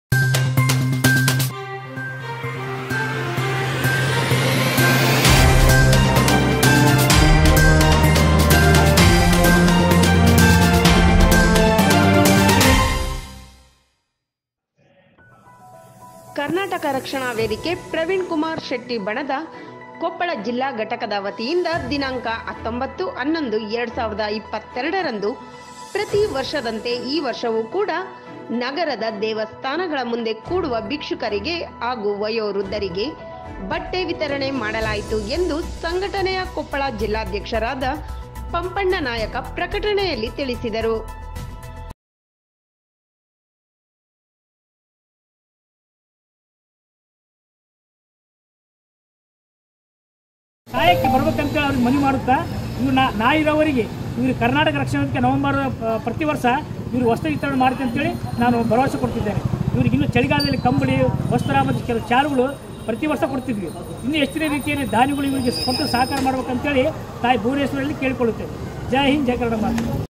ಕರವೇ ಪ್ರವೀಣಕುಮಾರ ಶೆಟ್ಟಿ ಬಣದ ಸಂಘಟನೆ ವತಿಯಿಂದವಯೋವೃದ್ಧರಿಗೆ ಬಟ್ಟೆ ವಿತರಣೆ ಮಾಡಿದ ಜಿಲ್ಲಾಧ್ಯಕ್ಷ ಪಂಪಣ್ಣ ನಾಯಕ.
ಗಂಗಾವತಿ: ಕರ್ನಾಟಕ ರಕ್ಷಣಾ ವೇದಿಕೆ ಪ್ರವೀಣಕುಮಾರ ಶೆಟ್ಟಿ ಬಣದ  ಕೊಪ್ಪಳ ಜಿಲ್ಲಾ ಘಟಕದ ವತಿಯಿಂದ ದಿನಾಂಕ: ೧೯.೧೧.೨೦೨೨ ರಂದು ಪ್ರತಿವರ್ಷದಂತೆ ಈ ವರ್ಷವೂ ಕೂಡಾ ನಗರದ ದೇವಸ್ಥಾನಗಳ ಮುಂದೆ ಕೂಡುವ ಭಿಕ್ಷಕರಿಗೆ ಹಾಗೂ ವಯೋ ವೃದ್ಧರಿಗೆ ಬಟ್ಟೆ ವಿತರಣೆ ಮಾಡಲಾಯಿತು.
ಎಂದು ಸಂಘಟನೆಯ ಕೊಪ್ಪಳ ಜಿಲ್ಲಾಧ್ಯಕ್ಷರಾದ ಪಂಪಣ್ಣ ನಾಯಕ ಪ್ರಕಟಣೆಯಲ್ಲಿ ತಿಳಿಸಿದರು.

ವೃದ್ಧರಿಗೆ ಬಟ್ಟೆ, ಸೀರೆ, ಪಂಚೆ, ಶಾಲು ವಿತರಣೆ ಮಾಡಿದ ಅವರು ಮಾತನಾಡಿ, ಕಳೆದ ೧೭ ವರ್ಷಗಳಿಂದ ಪ್ರತಿವರ್ಷ ನಮ್ಮ ಸಂಘಟನೆ ಈ ರೀತಿಯಲ್ಲಿ ವೃದ್ಧರಿಗೆ ಬಟ್ಟೆ ವಿತರಣೆ ಹಾಗೂ ಇನ್ನಿತರ ಸಾಮಾಜಿಕ ಕಳಕಳಿ ಕಾರ್ಯಕ್ರಮಗಳನ್ನು ಹಮ್ಮಿಕೊಳ್ಳಲಾಗುತ್ತಿದೆ. ಇಂದಿನ ಯುವಕರು ಹಿರಿಯ ನಾಗರಿಕರಿಗೆ, ತಂದೆ ತಾಯಿಗಳಿಗೆ ಗೌರವ ನೀಡಿ, ಅವರ ಪೋಷಣೆ ಮಾಡಲು ಕರೆ ನೀಡಿದರು.ಈ ಸಂದರ್ಭದಲ್ಲಿ ಕರವೇ ಸಂಘಟನೆಯ ಪದಾಧಿಕಾರಿಗಳಾದ ಶಂಕರ ಪೂಜಾರಿ, ಹಸೆಕಟಗಿ ಹನುಮೇಶ ನಾಯಕ, ಶರಣಪ್ಪ ನಾಯಕ ಪುಂಡಲೀಕ ಬಿಸಿಲದಿನ್ನಿ, ಹುಸೇನಸಾಬ, ಉಮೇಶ ಬಂಡಿ, ಅಂಜಿನಪ್ಪ ಪೂಜಾರಿ, ಹಸೇನಸಾಬ, ವೆಂಕಟೇಶ, ಪರಶುರಾಮ, ಭರಮಪ್ಪ, ಈರಣ್ಣ, ಹುಲುಗಪ್ಪ ಸೇರಿದಂತೆ ಇನ್ನಿತರರು ಪಾಲ್ಗೊಂಡಿದ್ದರು.

ವರದಿ : ಶರಣಪ್ಪ ಗಂಗಾವತಿ